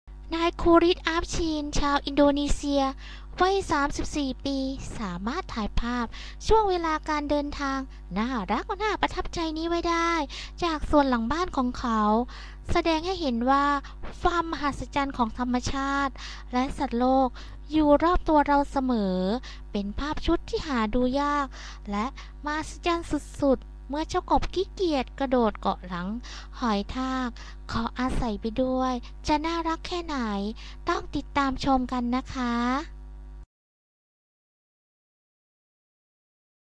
ภาพน่ารักน่าประทับใจเมื่อกบขี้เกียรติขี่หลังหอยทาก
ภาพน่ารักน่าประทับใจเมื่อกบขี่เกียรติขี่หลังหอยทาก มาดูภาพที่หาดูยากกัน ว่าภาพนั้นจะน่ารักมากน้อยแค่ไหน